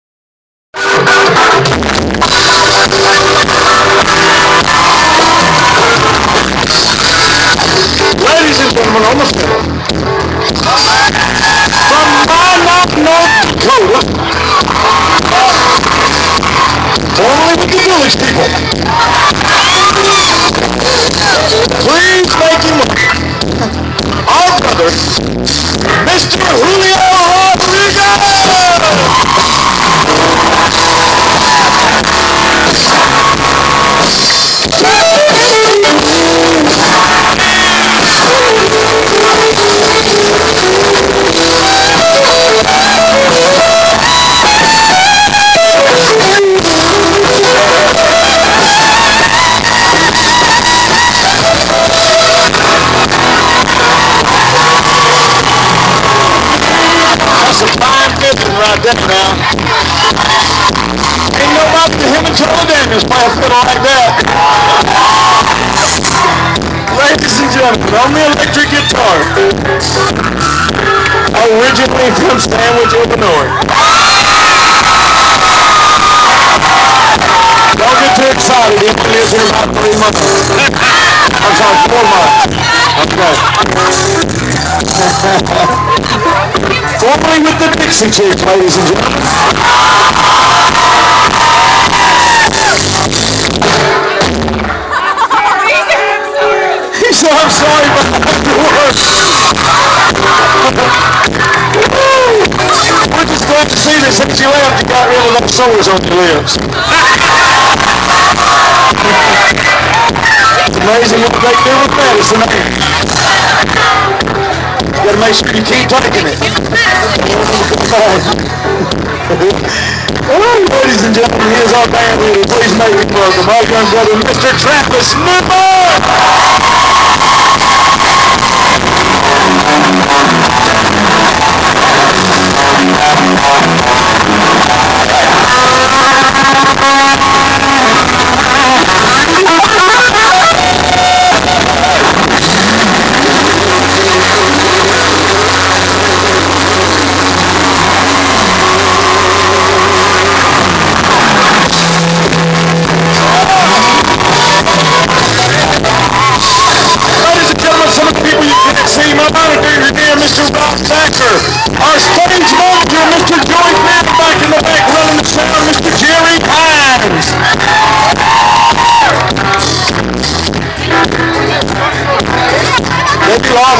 Craig Morgan - Sandwich Fair - September 9, 2011, Part 2
Craig introduces his band and crew